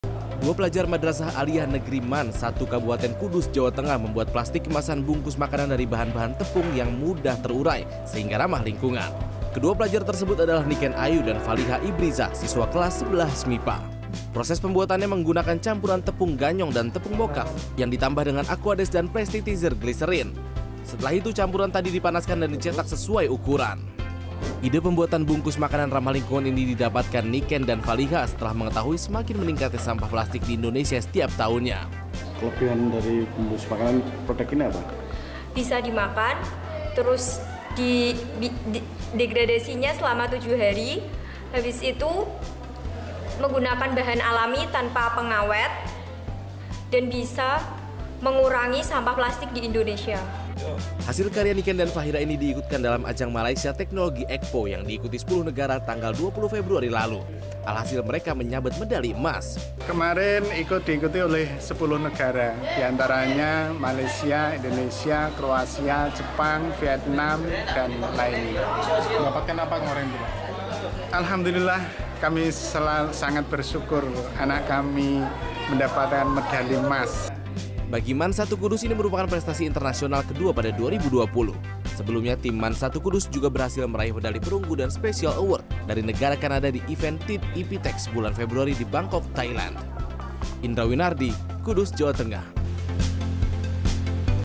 Inovasi Bungkus Makanan Bisa Dimakan
Prihatin dengan menumpuknya sampah plastik yang sulit diurai, dua pelajar Madrasah Aliyah, di  Kudus,  Jawa Tengah, berinisiatif membuat kemasan makanan yang ramah lingkungan. Ya, Kemasan plastik untuk bungkus makanan tersebut dibuat dari campuran tepung, dan bisa dikonsumsi.

Like & Follow Facebook:
CNN Indonesia

cnnindonesiatv